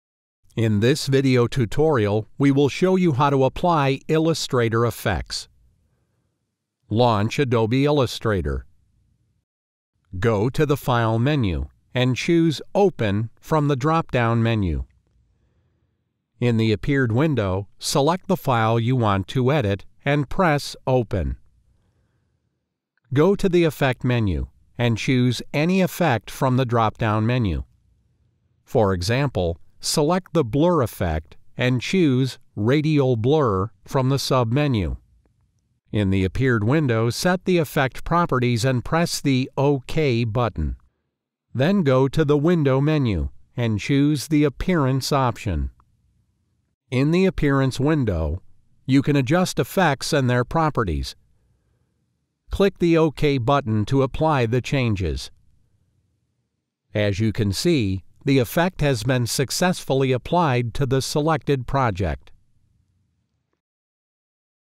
How to Apply Effects in Adobe Illustrator CS6
Learn how to apply some effects to the objects in the Adobe Illustrator

In this video tutorial we will show you how to apply Illustrator effects.

Launch the Adobe Illustrator. Go to the "File" menu and choose "Open" from the drop-down menu.

In the appeared window select the file you want to edit and press "Open". Go to the "Effect" menu and choose any effect from the drop-down menu.

For example, select the "Blur" effect and choose "Radial Blur" from the submenu.

In the appeared window set the effect properties and press the "OK" button.  Then go to the "Window" menu and choose the "Appearance" option.

In the "appearance window" you can adjust effects and their properties. Click the "OK" button to apply changes. As you can see, the effect has been successfully applied to the selected object.